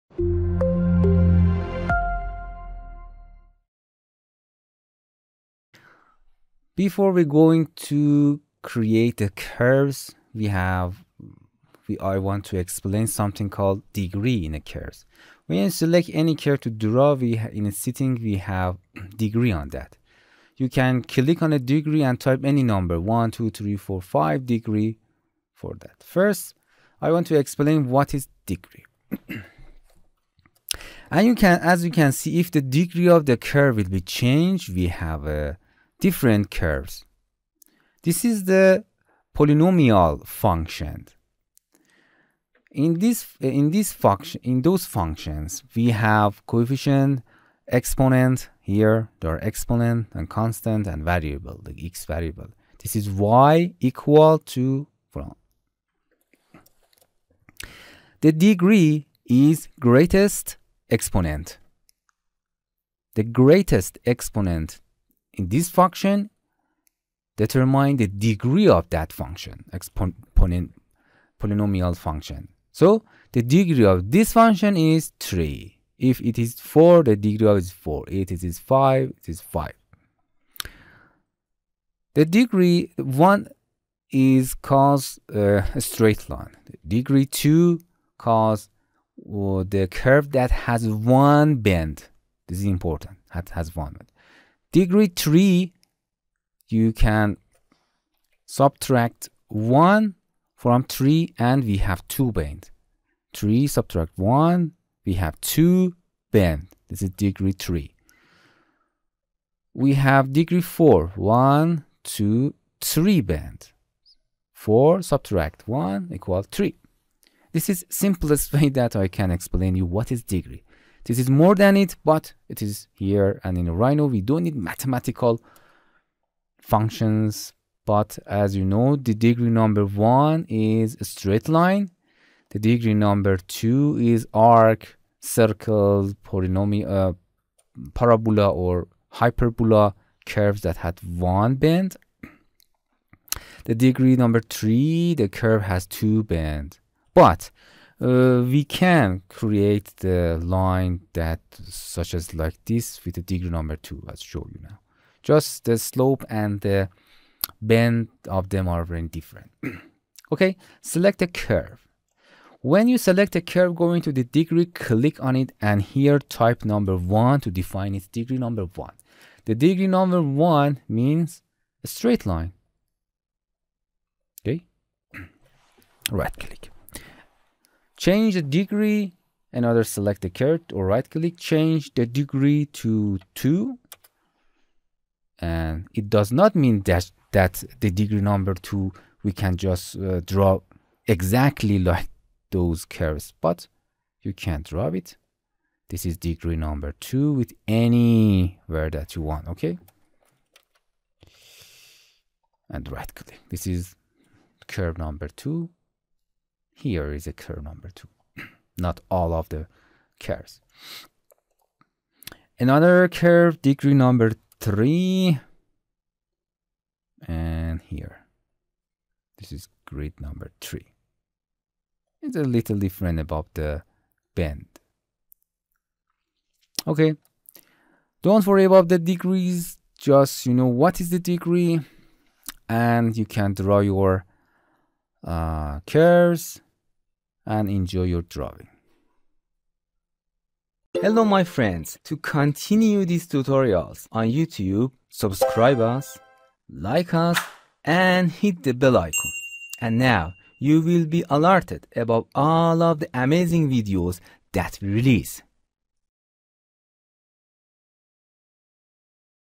0046. curve degree in rhino
In this lesson we want to get acquainted with the concept of curve degree. We have types of curves with degrees 1, 2 and 3. You can set the degree of curves and lines when rhyming.